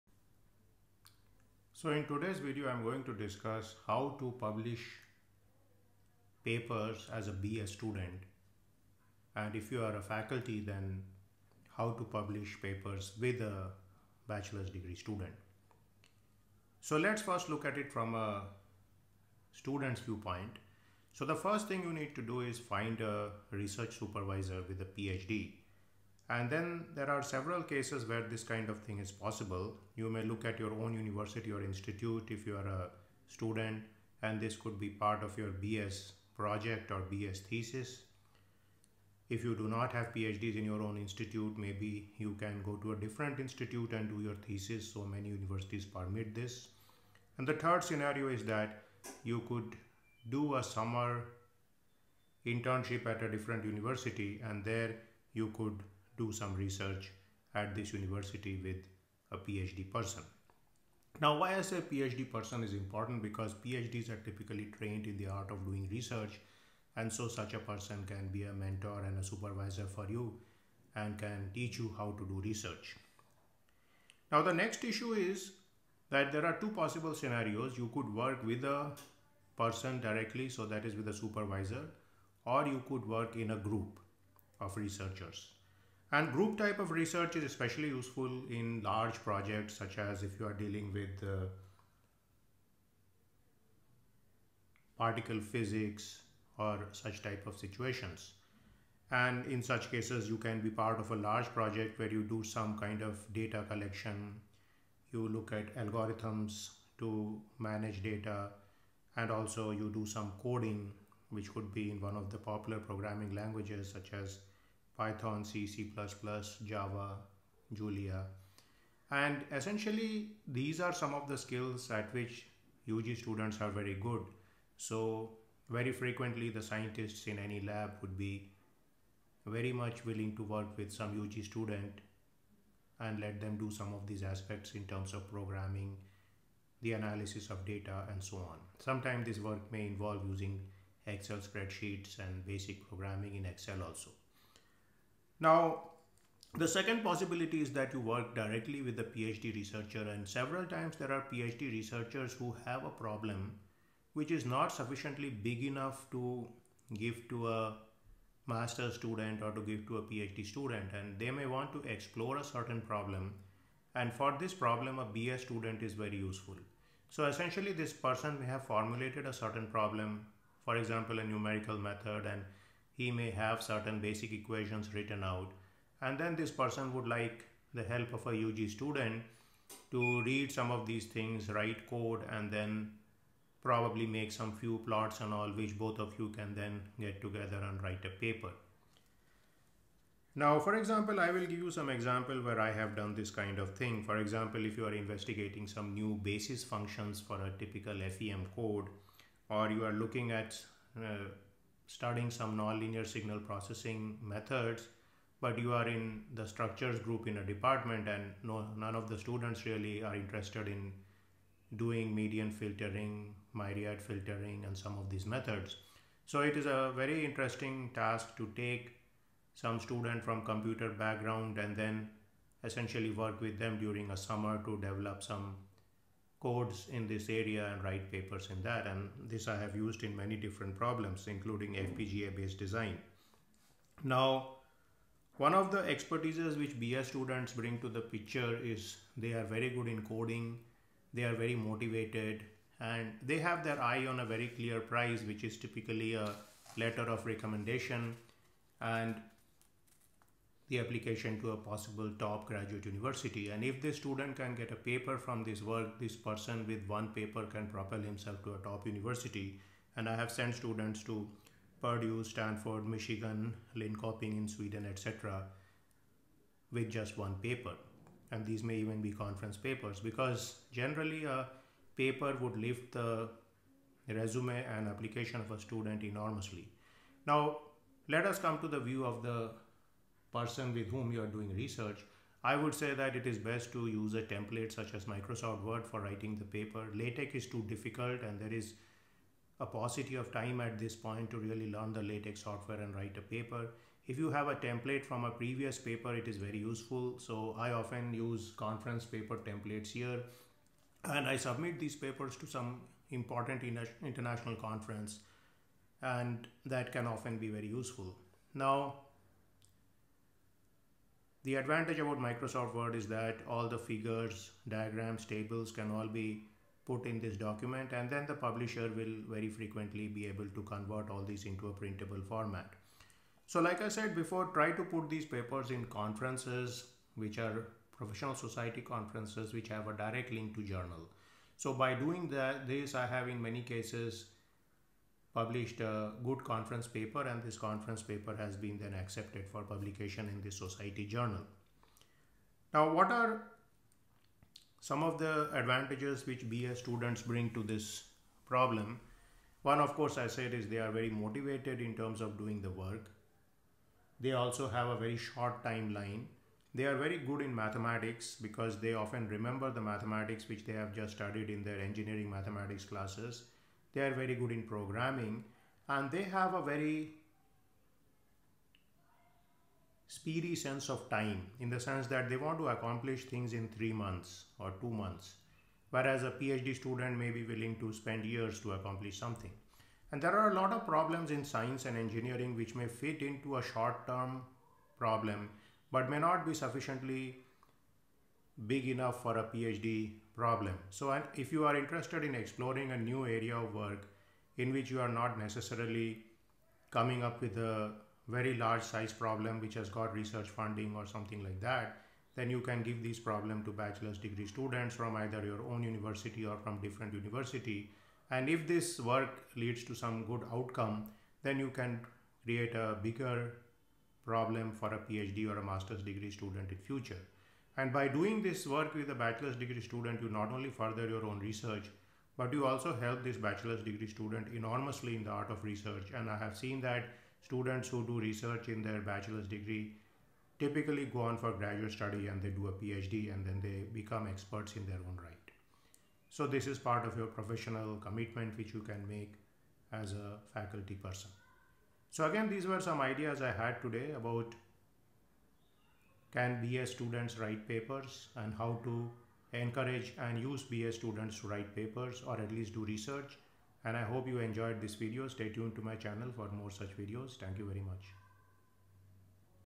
How to write a research paper during bachelor’s degree?
College students can write research papers and faculty and researchers can advice them in this endeavor. Such a paper can help students in getting a scholarship for the Master's degree and also contribute to the research effort of the group hosting this undergraduate student as a summer intern. There is scope of going abroad for summer internship for undergrad students where they can work with good research groups and be part of a conference or journal paper.